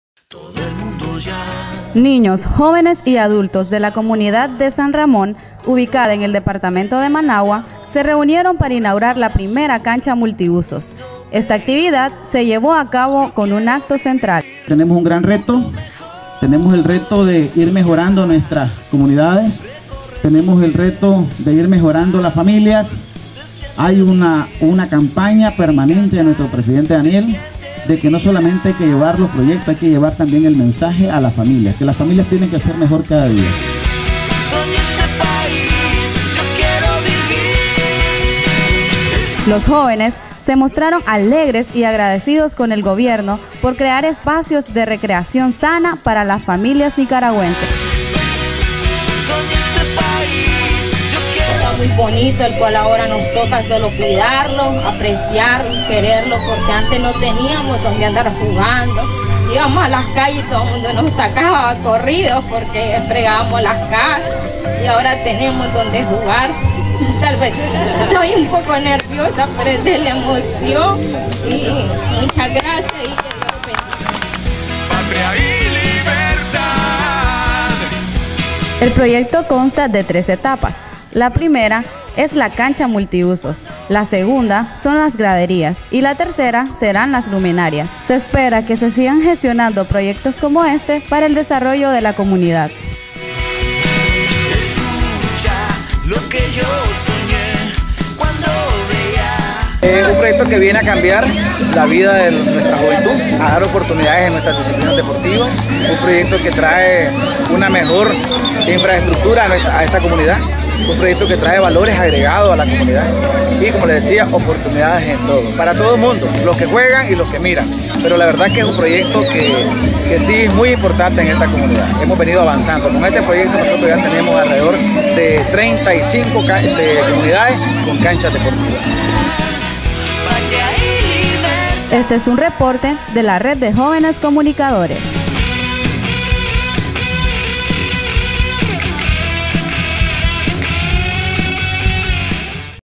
INAUGURACIÓN DE CANCHA MULTIUSO - SAN FRANCISCO LIBRE
Alcaldía y miembros de la comunidad de San Ramón, departamento de Managua, participaron en la inauguración de la primera cancha multiuso, para la recreación sana de niños, jóvenes y adultos y su desarrollo en el deporte.